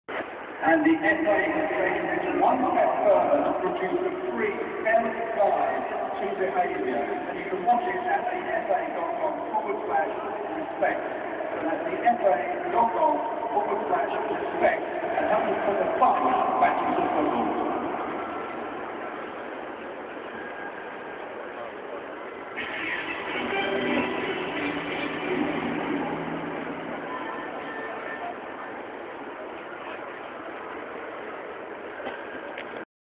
Inside wembley
this is short video of inside wmebley befoe England vs Slovakia